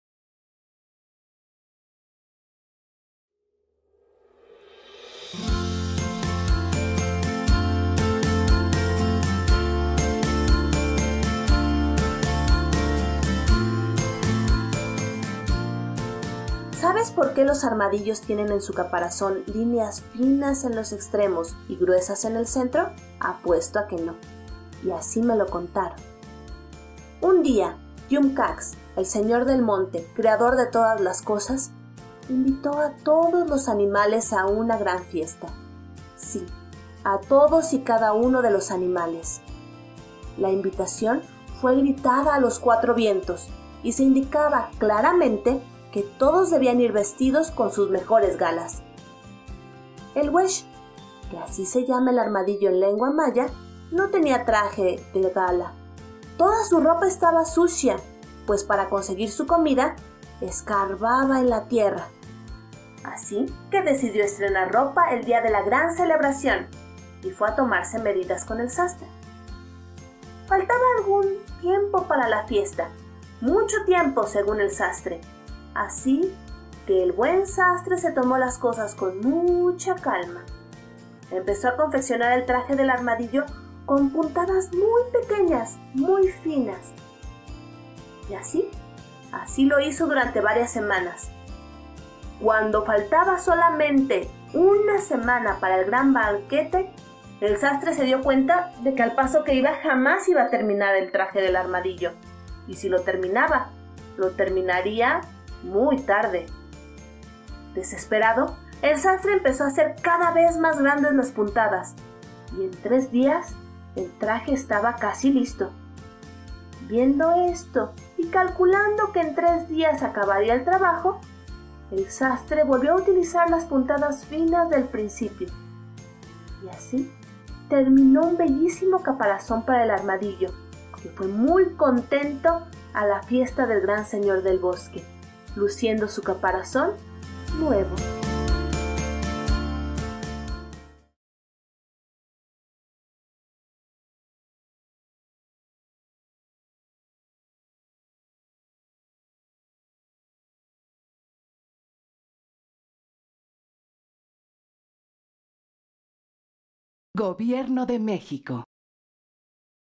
Cuento: El traje del armadillo
Gabriela Ladrón de Guevara narra el cuento "El traje del armadillo", que aparece en el libro "Así cuentan y juegan en el Mayab (CONAFE, México, 2000).